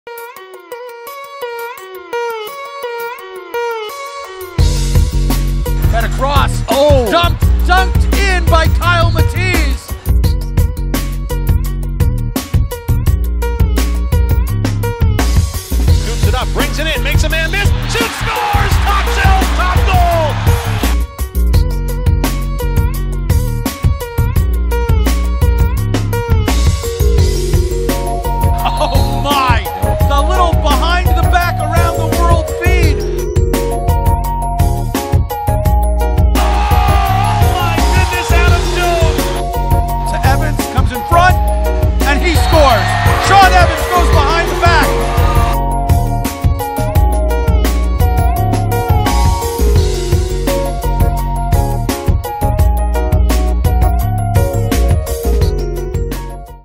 Top 5 Plays | NLL 2017 Week 15 Highlights | StringKing Lacrosse
Check out our Top 5 goals from last week's NLL action! Which one is your favorite? That BTB feed to Johnny Powless on #3 was ridiculous! And don't forget, you can catch all the action live right on NLLtv.com.

The official lacrosse head, shaft, & mesh supplier of the NLL & MLL.
The #1 mesh in college lacrosse.